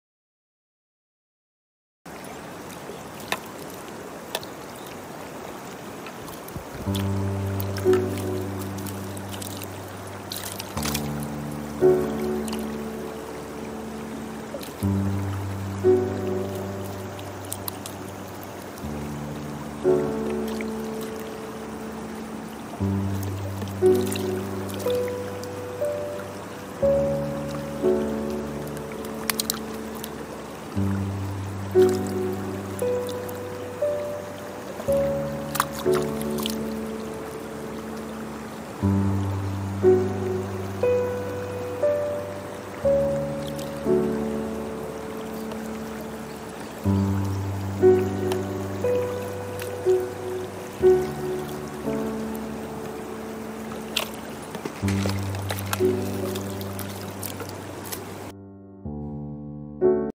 【渓流釣り ほうき川】7/23 餌釣り関谷付近 # short river Healing Channel （リバヒ）
ショート動画　箒川,山女魚、タイミング合えば大物がヒットすることもあります。